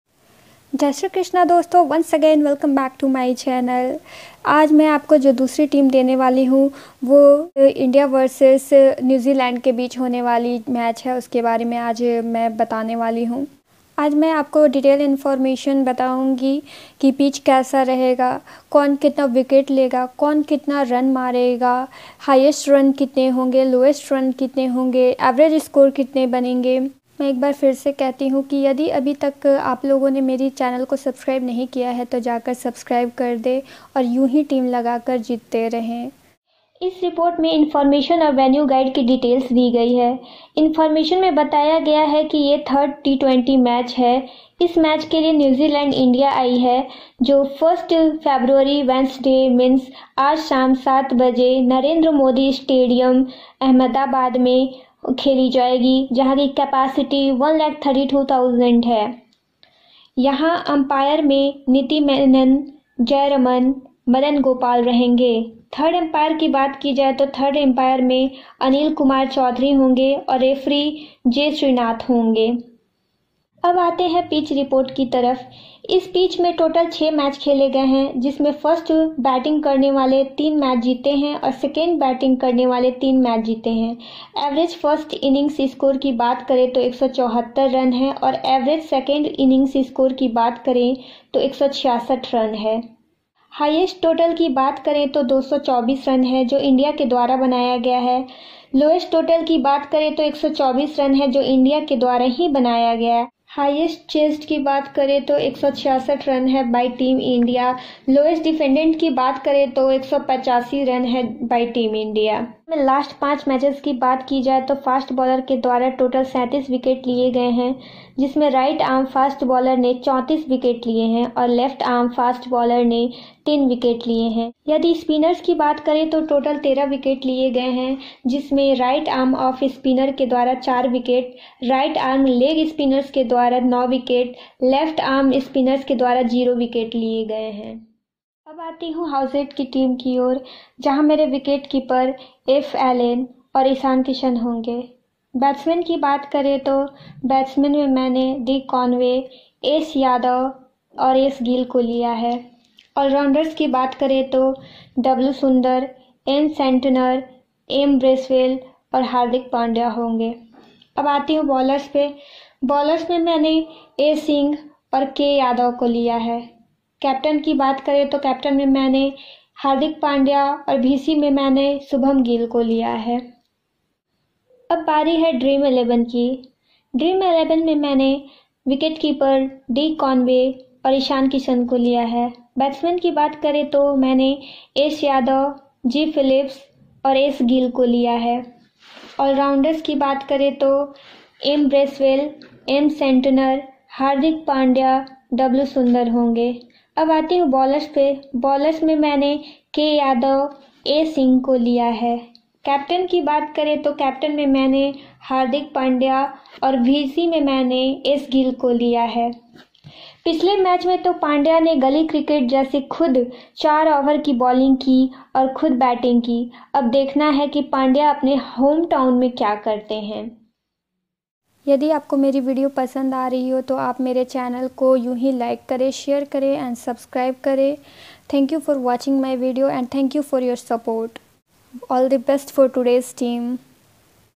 आज होंगे पैसों की बारिश! T20 IND VS NZ
वीडियो में बताया गया है कि कहां मैच होगा, कैसा पिच रहेगा, कितना रन बनेगा, कौन कितना विकेट लेगा और भी जानकारी है इसमें